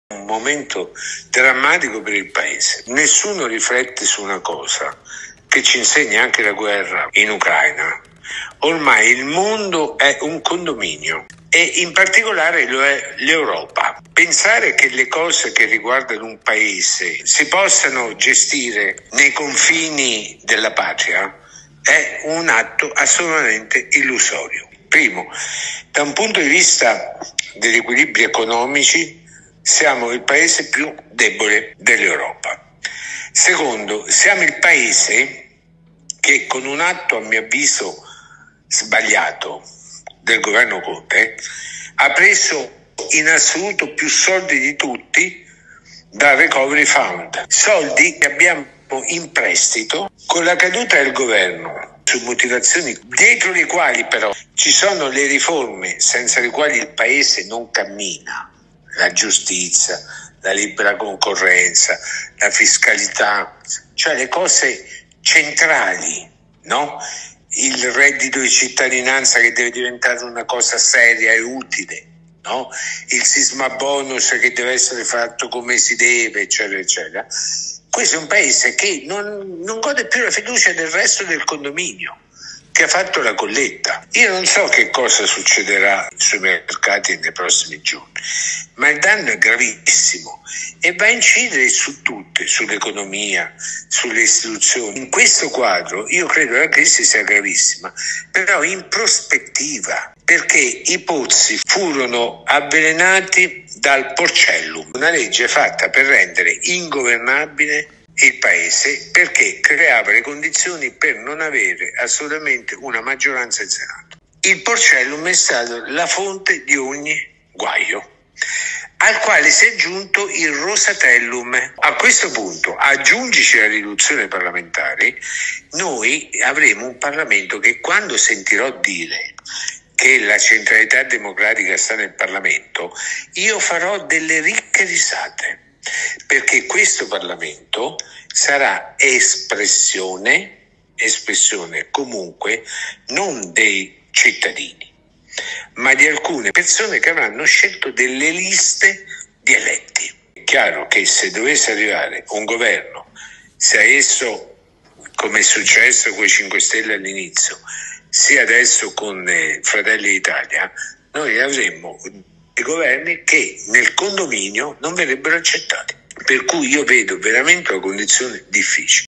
Cialente, è un momento drammatico per il Paese
L'ex sindaco dell'Aquila, Massimo Cialente commenta, con una preoccupazione rivolta al futuro, la situazione politica italiana generata dalla caduta del Governo Draghi.